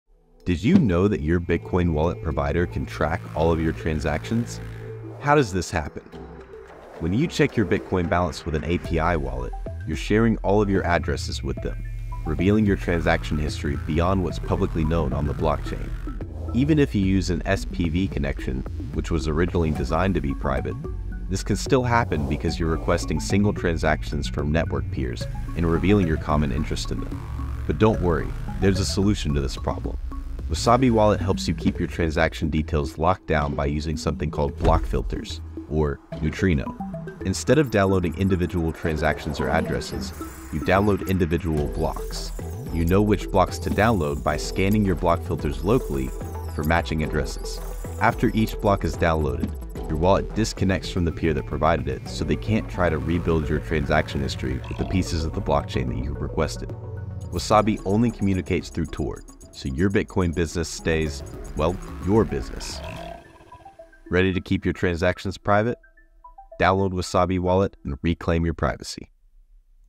Network Level Privacy For Your Bitcoin
Bitcoin light wallets have a privacy issue.
API and SPV wallets allow third party servers to collect data from you when sending or receiving transactions.
But… this can be solved!